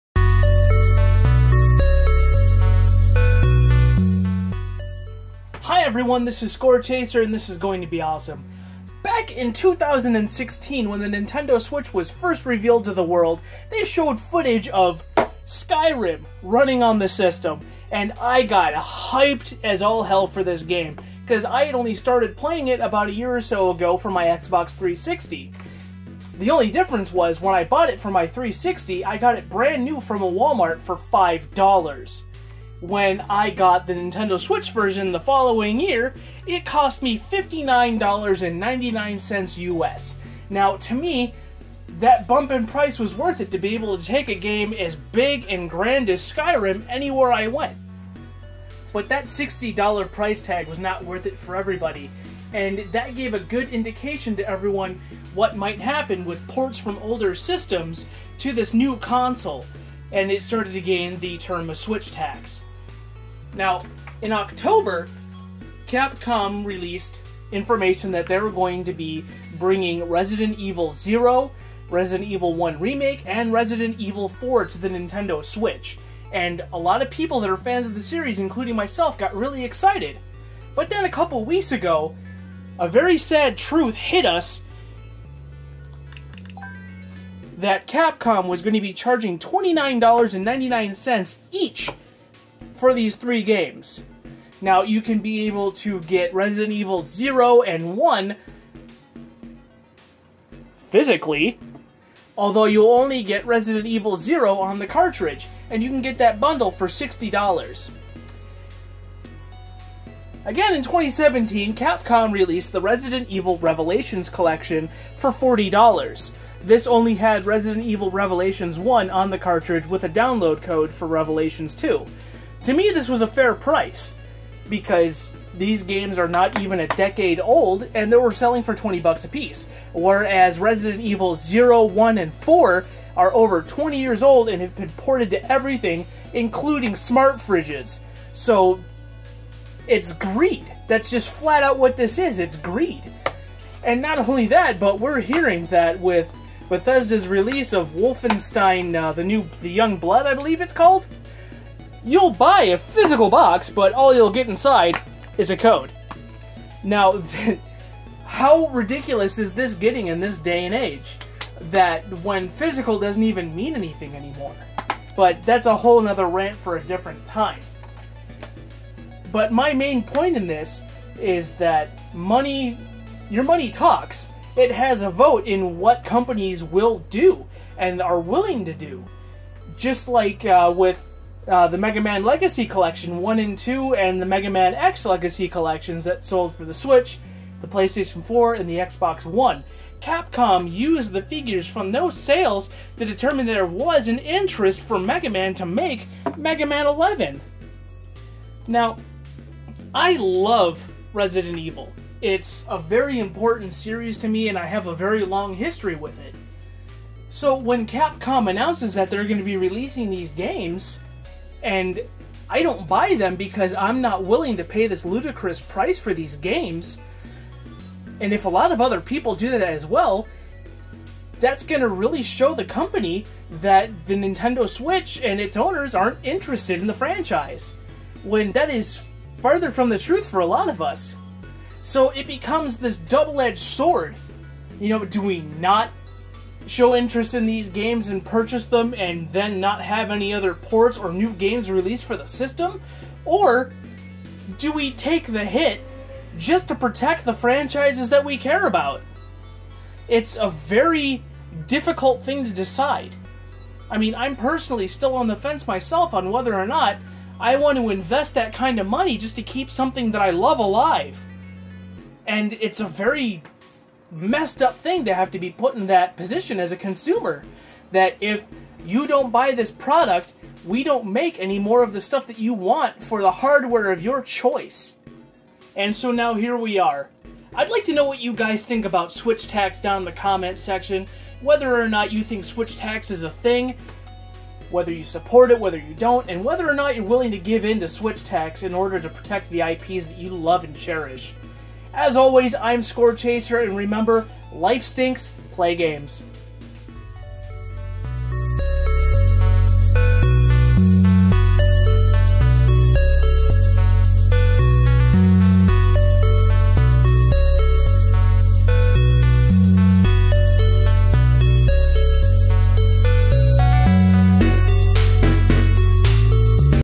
Switchtax
Join me as I rant about 1st world problems of paying more for video games on Nintendo Switc.